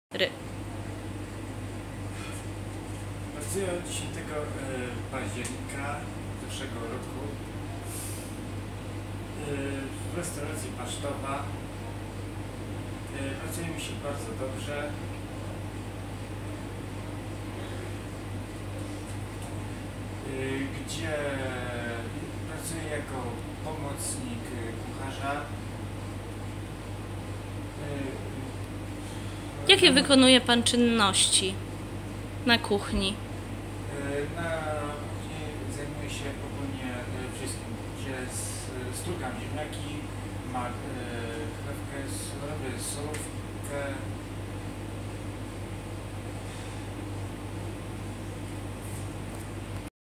Projekt PRACA URZECZYWISTNIENIEM SIEBIE_2
Uczestnicy projektu PRACA URZECZYWISTNIENIEM SIEBIE realizujący swoje obowiązki w pracy